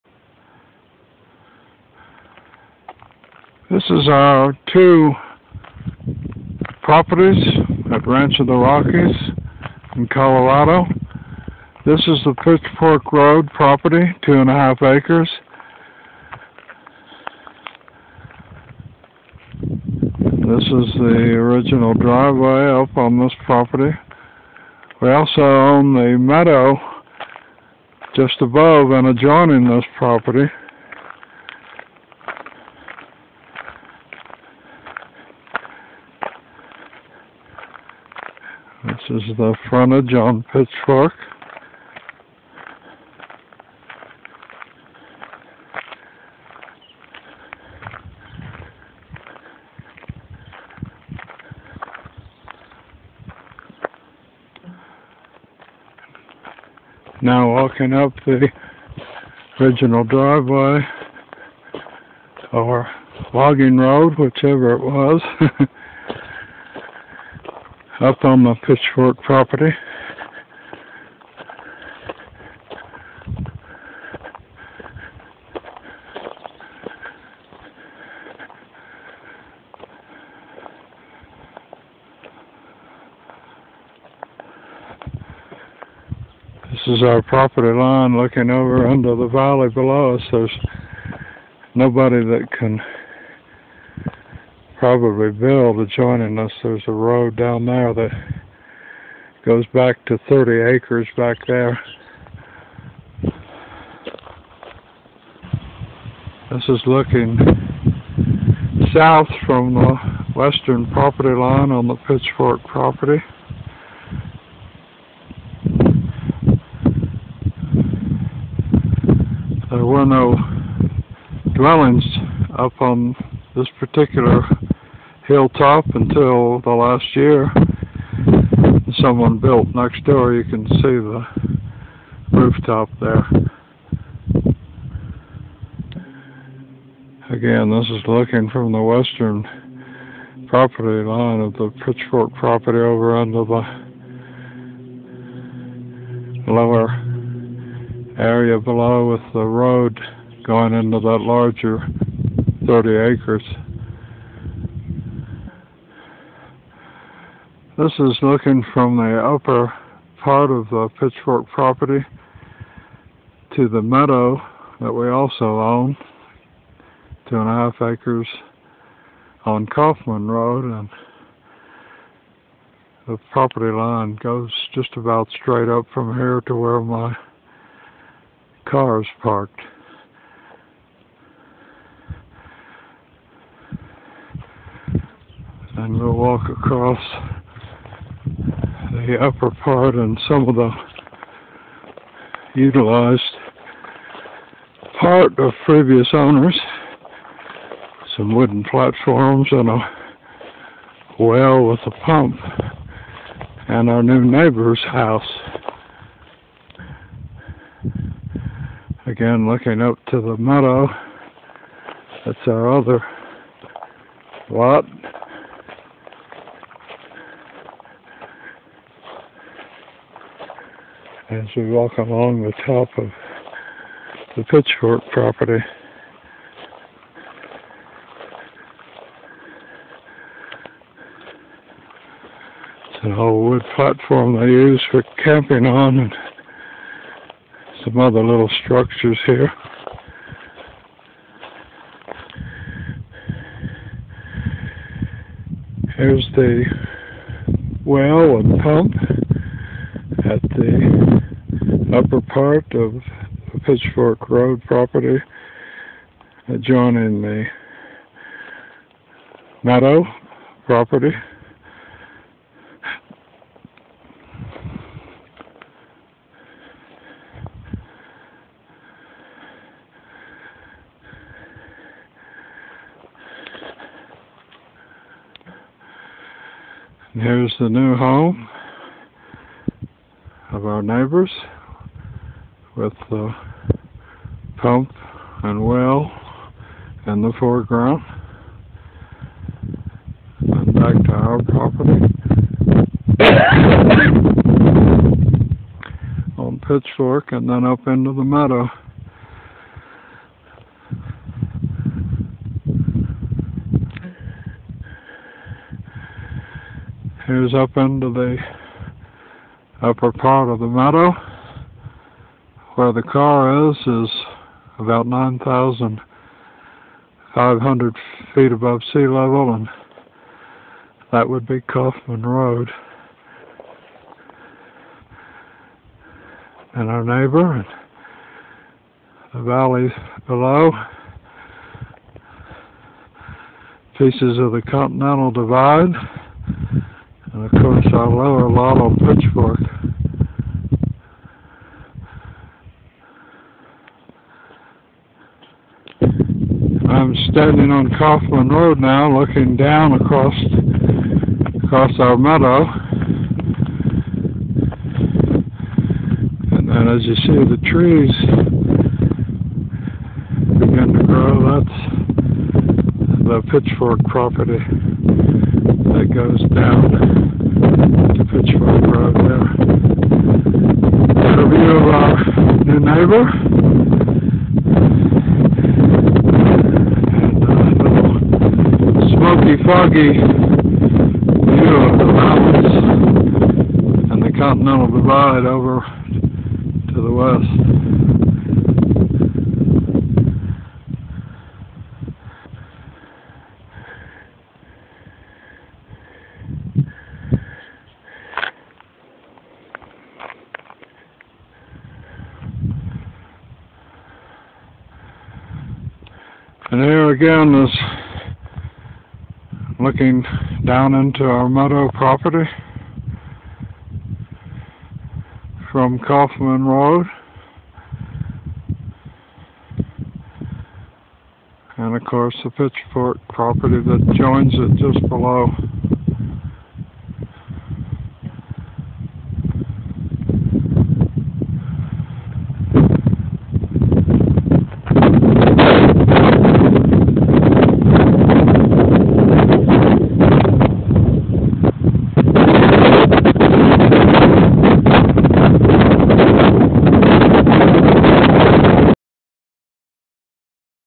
Pine Lots at ROR, CO.mp4
2-lots totaling 5.5 Acres at the Ranch of the Rockies, CO, 9,500-ft. elevation.